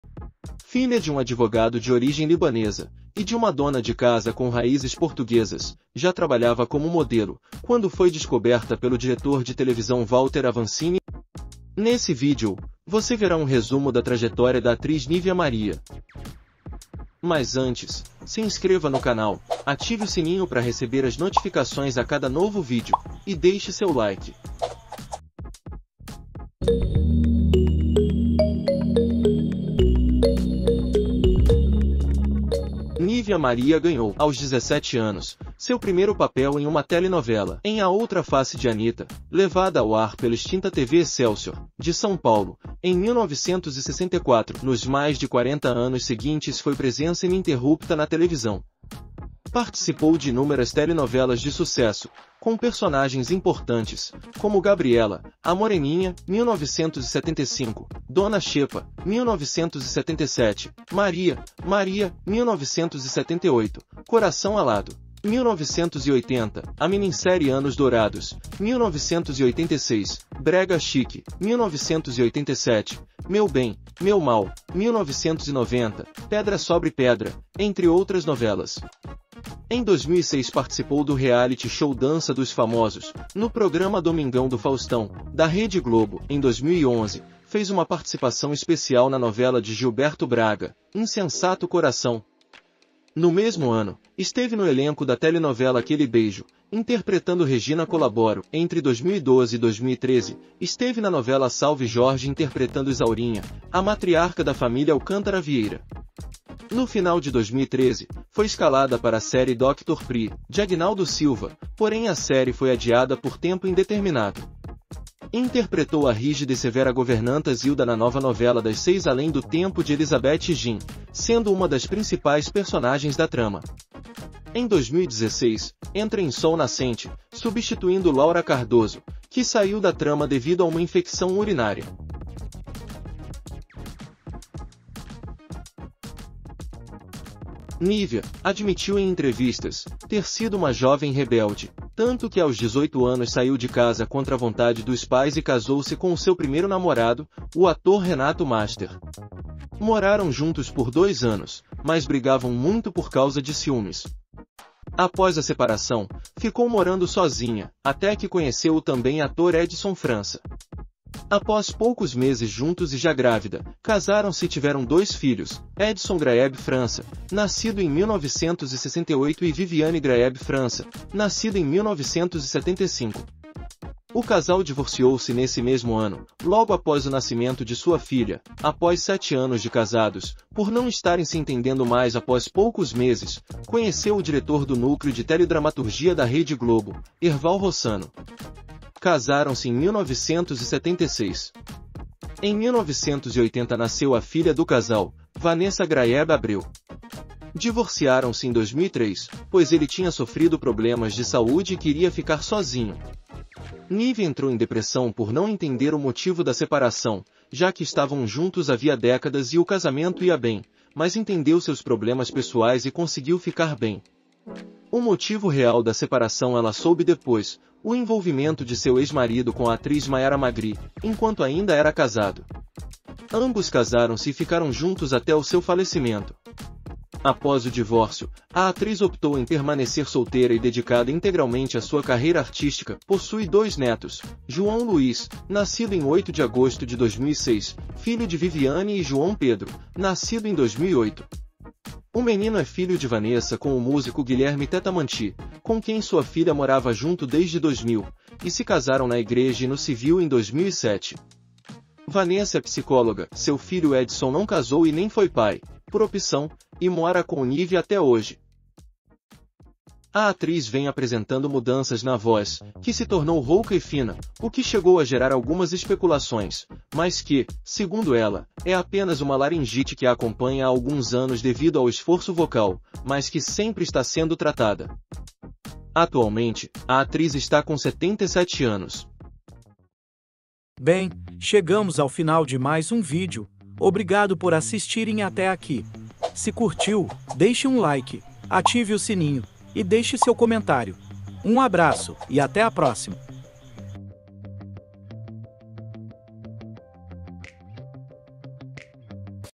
Conheça a história da atriz NÍVEA MARIA: Marido a deixou pela atriz Mayára Magri!
Veja aqui a trajetória da atriz NÍVEA MARIA. sua infância, carreira, relacionamentos, saiba quem foi seu grande amor e porque ele a deixou pra ficar com outra atriz.